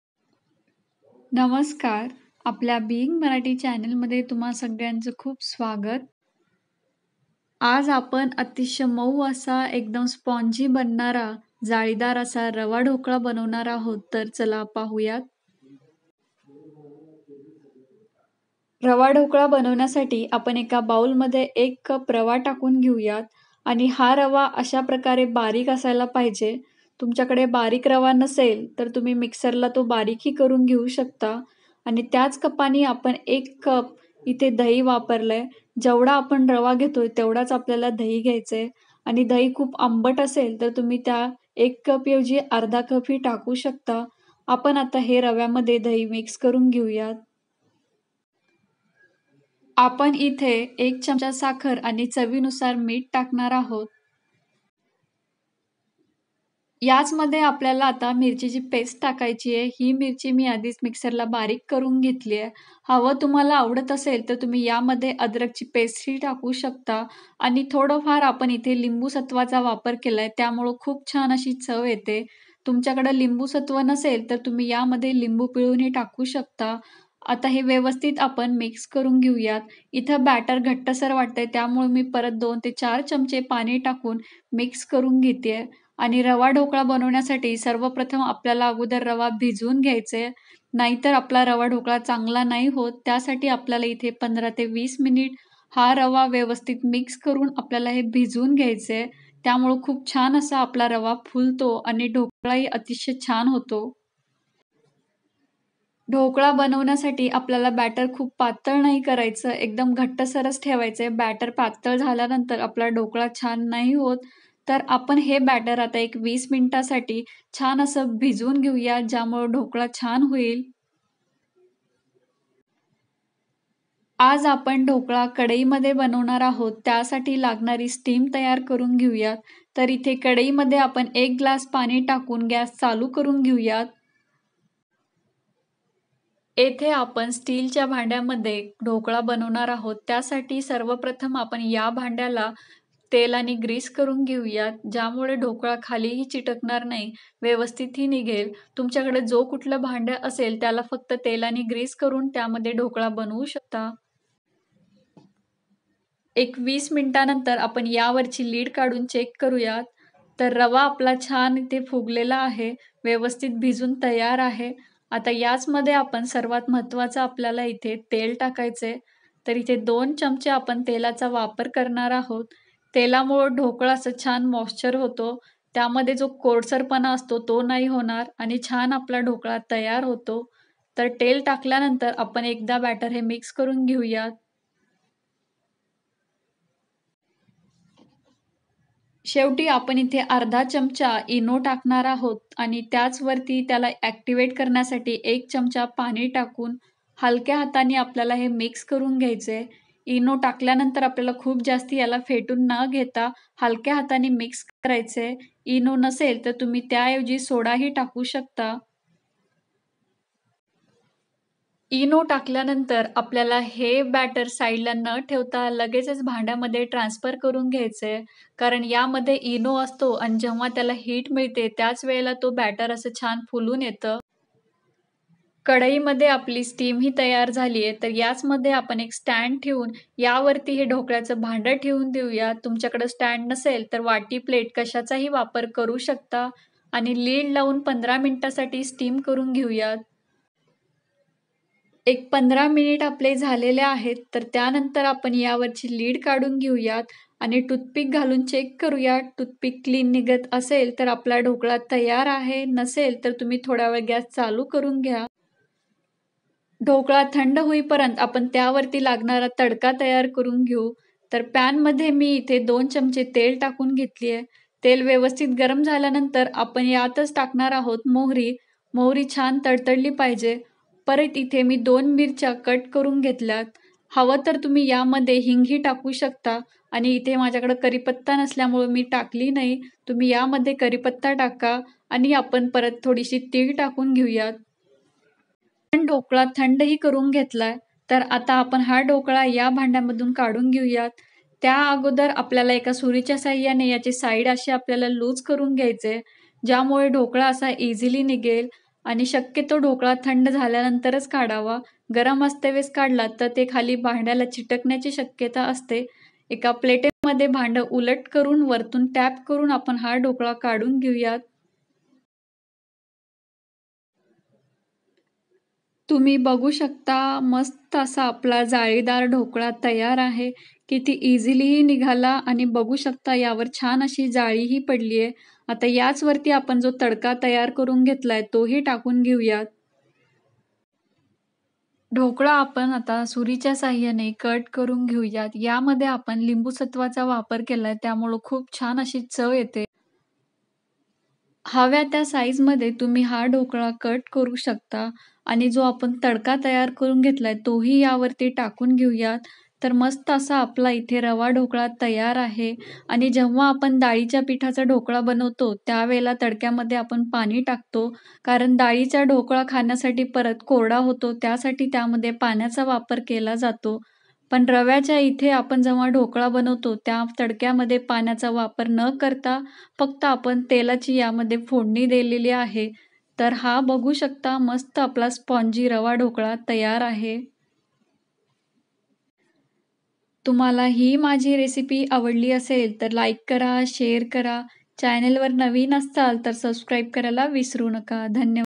या टिप्स लक्षात ठेवून बनवा एकदम मऊ लुसलुशीत रवा ढोकळा रेसिपी मराठी|rava dhokla|suji dhokla|semolina
ravadhokla AshwinisRecipe Marathi